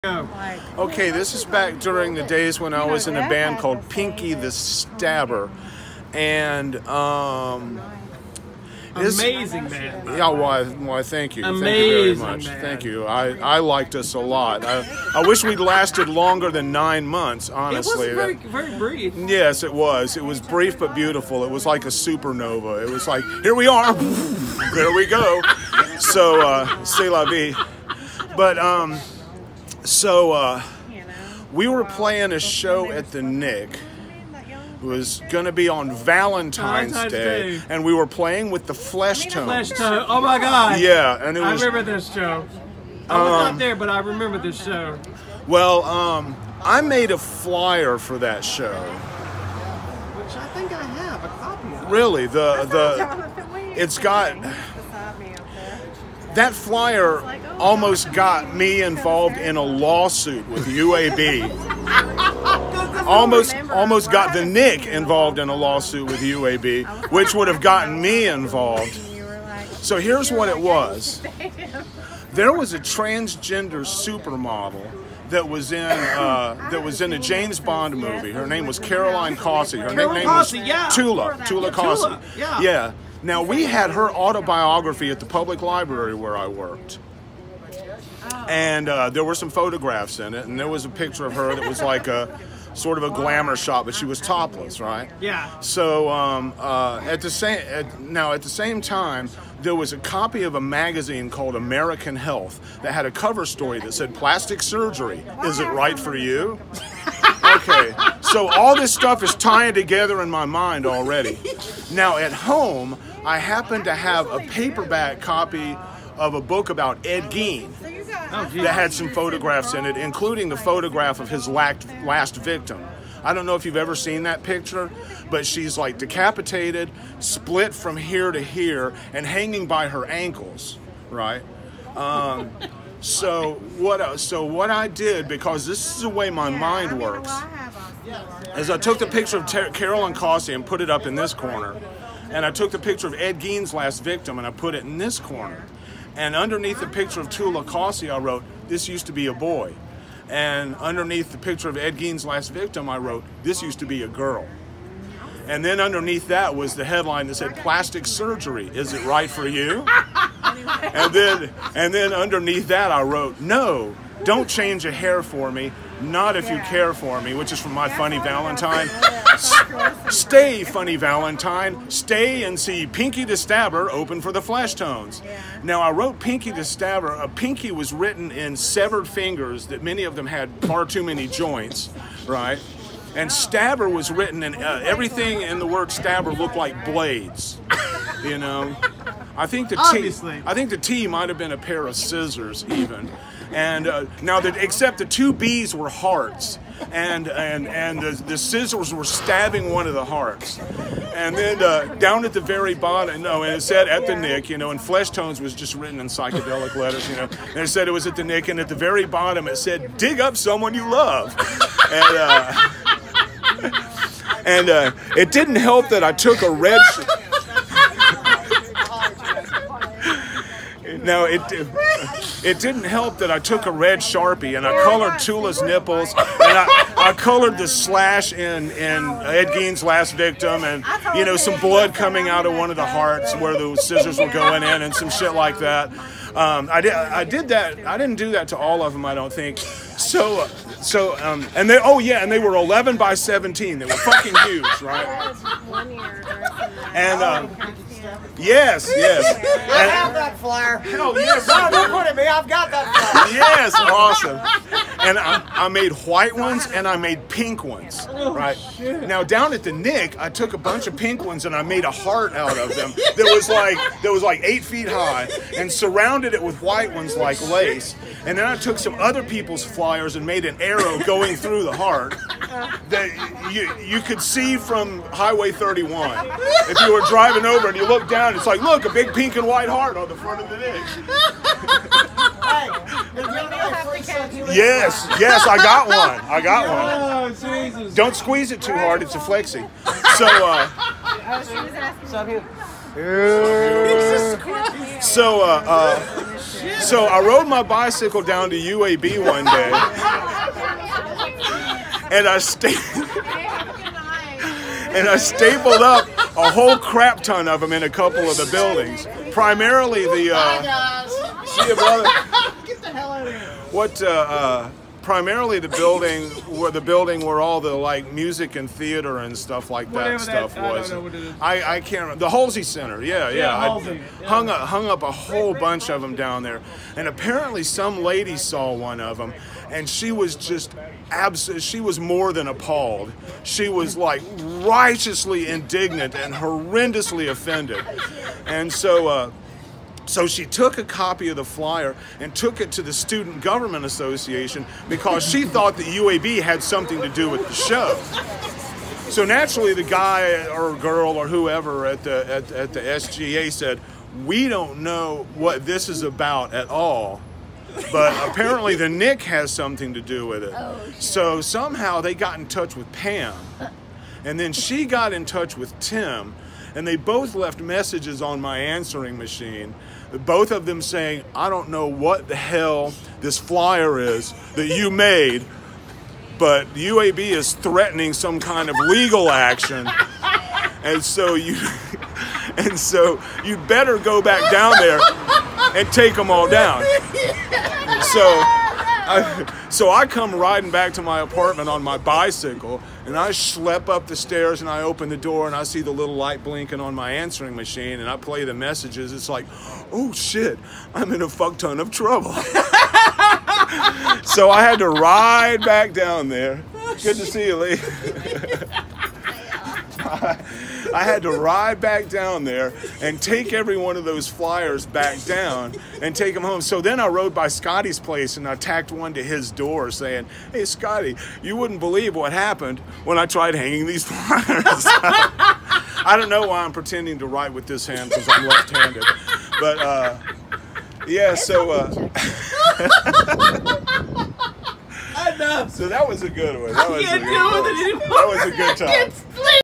Drunk Birmingham Music History with Christopher Dann Hendrix. Alabama Punk Rock Royalty!
“Pinky the Stabber and the UAB lawsuit”